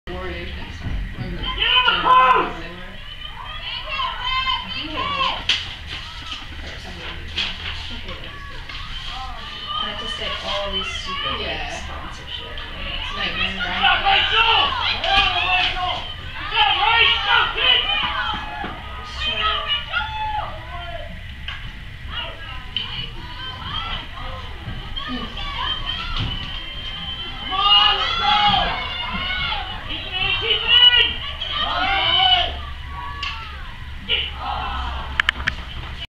Glenelg vs. Easton (Field Hockey) MD 2A State Semi-finals 11-5-12-2
First half action.